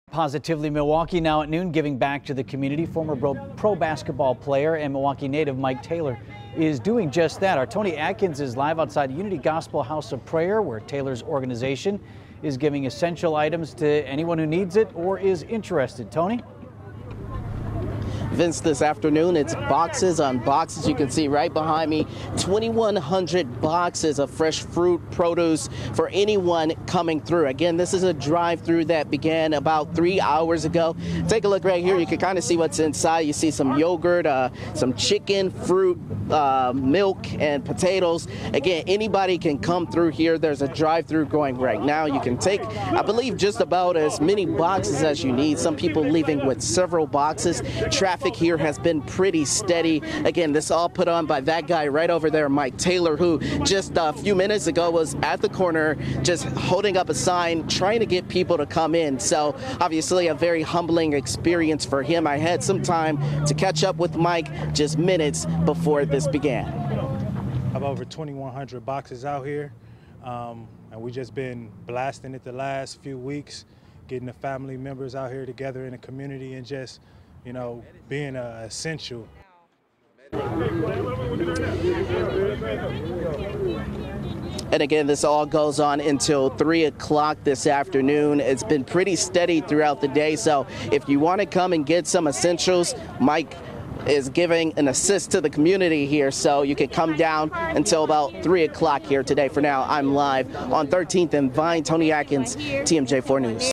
Former Pro Basketball player helps those in need
Former pro basketball player Mike Taylor helped out those in need on Monday.